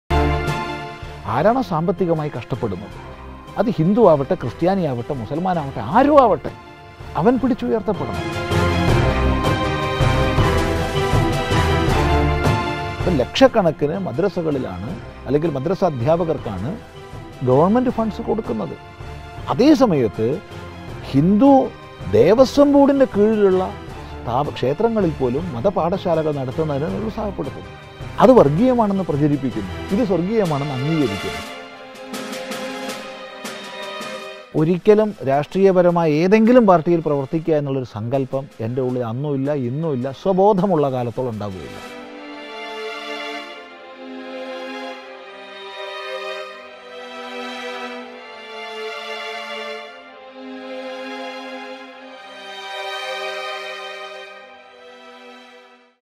തുറന്നു പറഞ്ഞ്........... സ്വാമി ചിദാനന്ദപുരി ഭാഗം 2 (20 -12 -2019 )വെള്ളിയാഴ്ച 6 pm
നിലപാടുകൾ തുറന്നു പറഞ്ഞു സ്വാമി ചിതാനന്ദപുരി
ഭാഗം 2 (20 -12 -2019 )വെള്ളിയാഴ്ച 6 pm